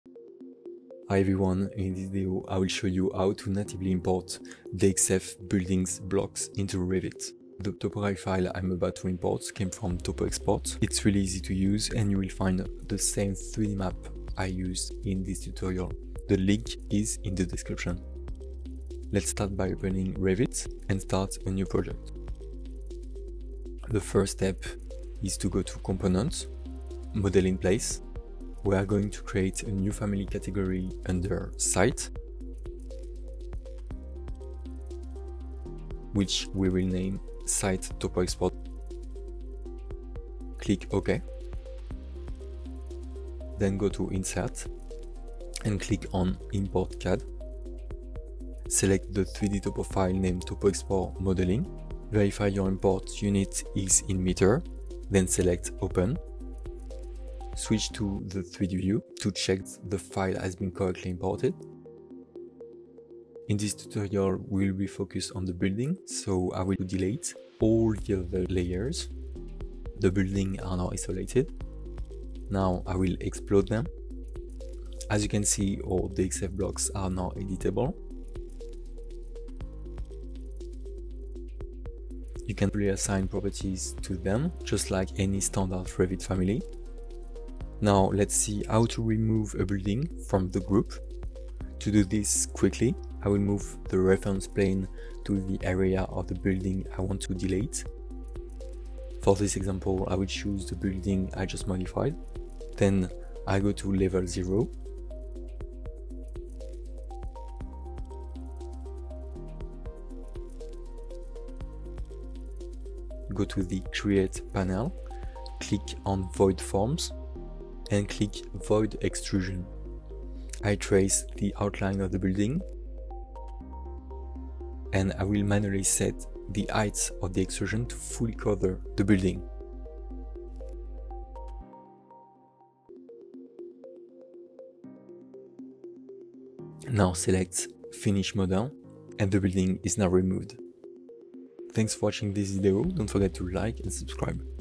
Revit - Import and Edit DXF Building Blocks from TopoExport (Native Workflow)
Learn how to natively import and edit DXF building blocks in Revit using data from TopoExport.com.
This step by step tutorial shows you how to bring in detailed 3D maps isolate buildings explode blocks and customize geometry directly in Revit without losing fidelity.